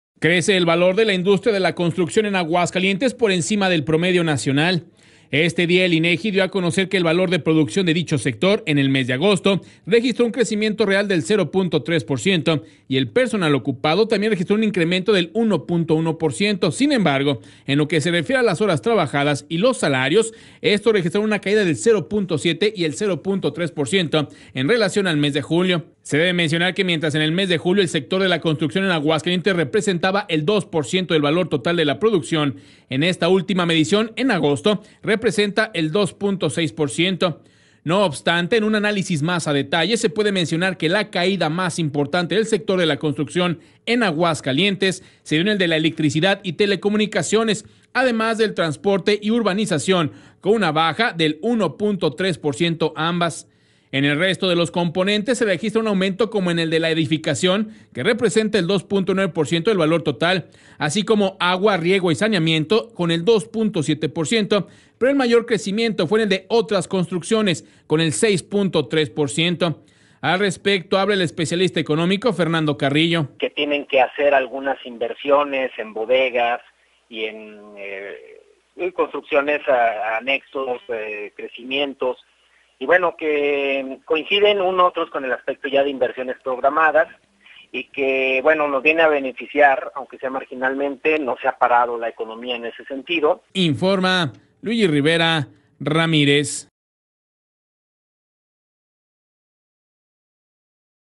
Bajo crecimiento de la construcción en Ags. por Fernando Carrillo en BITV
Entrevista de Luigi Rivera a Fernando Carrillo para Noticieros Radio Grupo, con motivo de los últimos datos presentados por el INEGI sobre el mes de agosto, respecto a indicadores de los sectores de la construcción durante lo que fue parte del 2016.